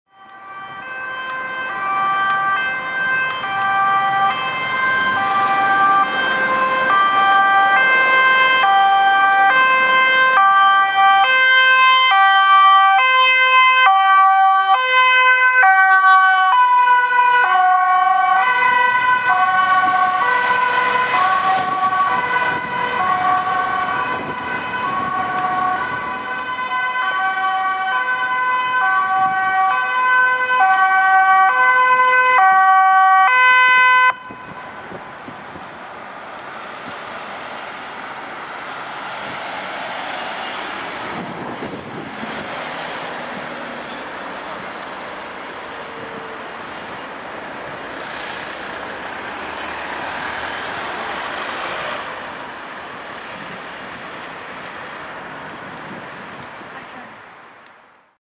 SPOEDAANKOMST 15-104 Bij EMCR na melding Rijswijk
Ambulance 15-104 met spoed naar EMCr na melding Rijswijk